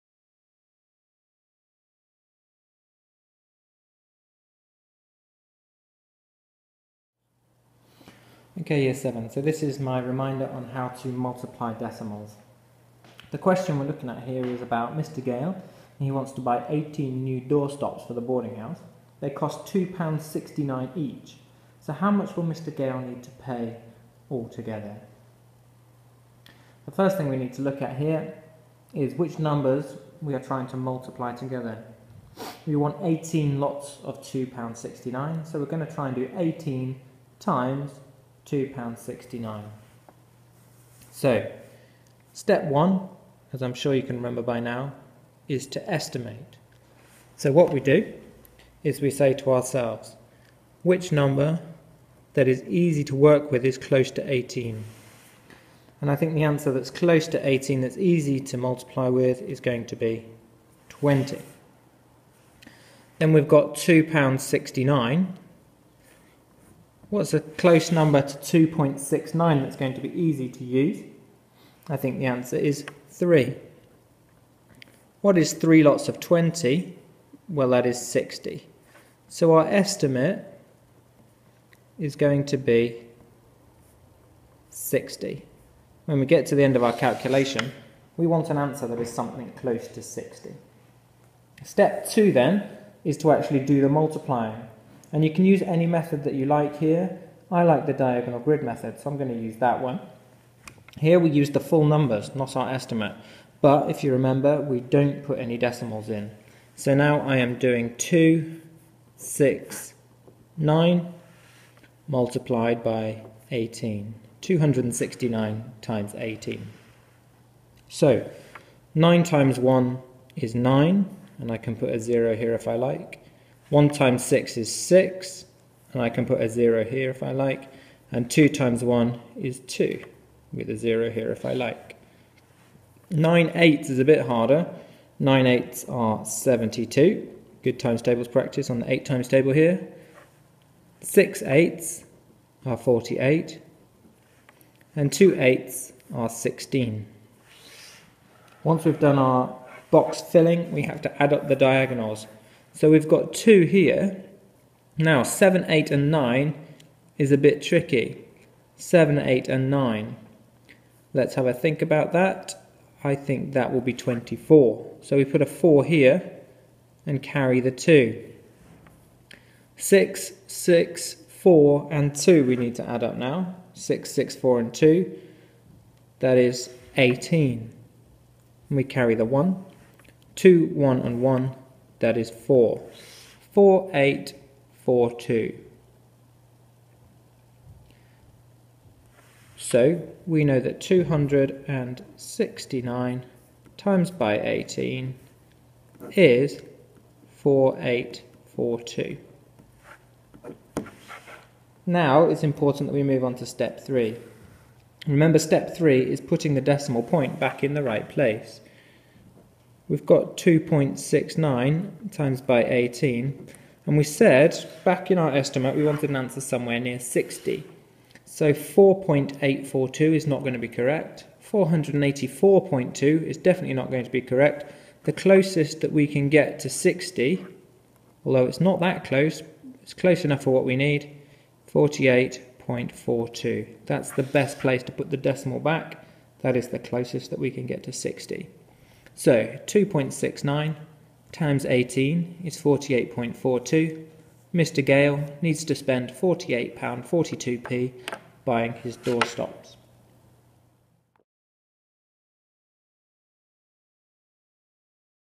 Multiplying Decimals Example - Year 7
This is an example of multiplying decimals.
It uses Mr. Hubbard's method of estimating before calculating.